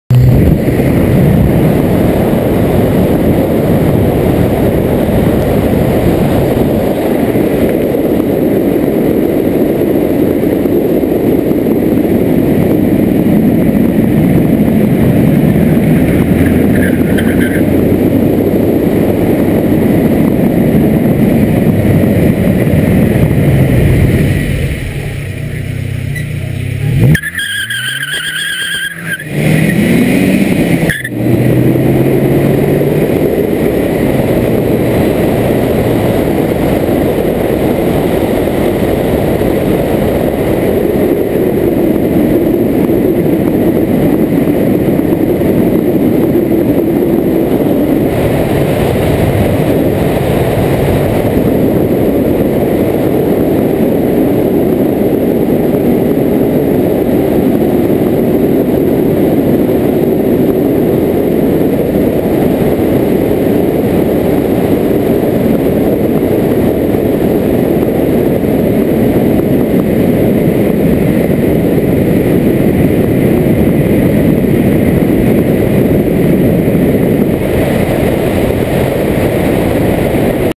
1990 vw jetta drive
buddy mikes 1990 vw jetta 1.8l 5 speed driving to aap